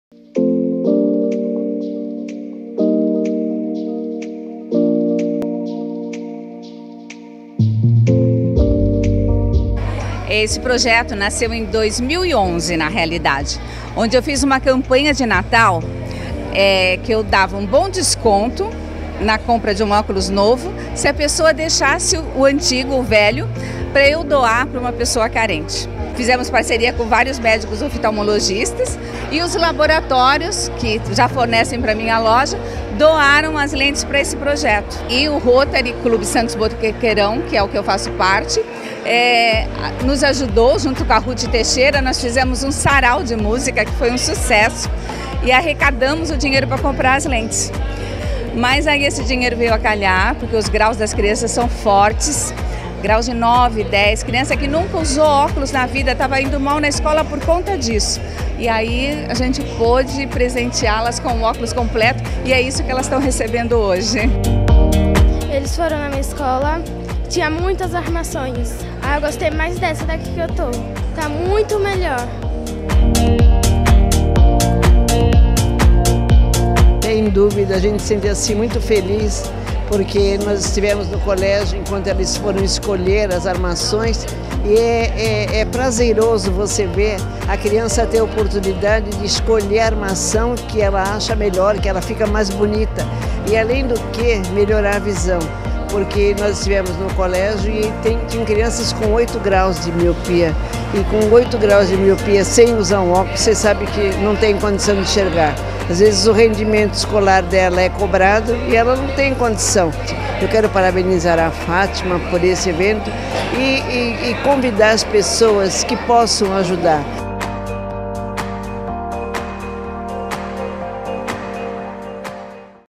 Olhar Solidário beneficia alunos da rede municipal
Ao longo do ano, mais de 600 alunos passaram por exames de vista e, destes, 70 tiveram indicação para passar por consulta na van oftalmológica da Saúde. No total, 38 tiveram indicação médica para usar óculos. A iniciativa faz parte do Projeto Olhar Solidário, realizado pela Donna Ótica, com apoio do Rotary Club Santos Boqueirão em parceria com o Fundo Social de Solidariedade (FSS) e com as secretarias de Saúde e de Educação, por meio do programa Saúde na Escola. A proposta pretende beneficiar pessoas de baixo poder aquisitivo com receita médica atualizada, armações e lentes. Saiba +